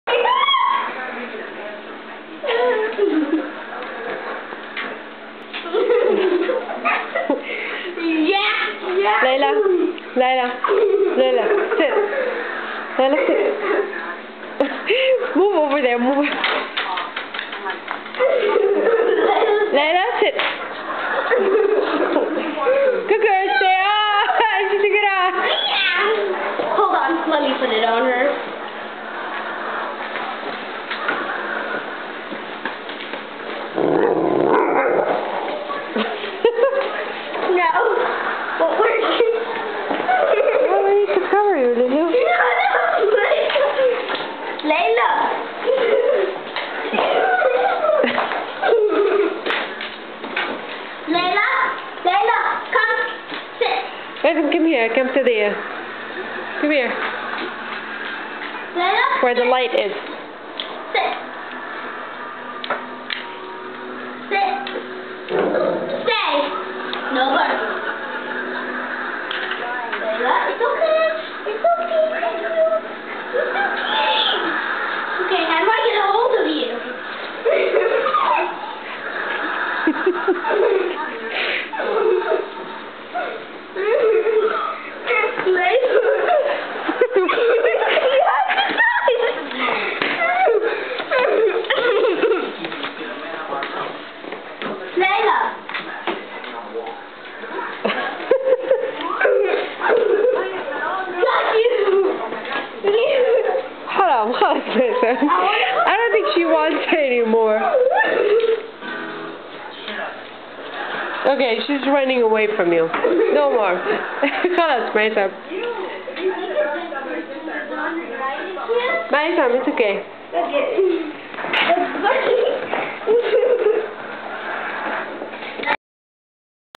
Great Dane dog wearing jacket
Maisam; my 9yr nephew, playing and dressing up Layla; Great Dane dog, a jacket. they're both adorable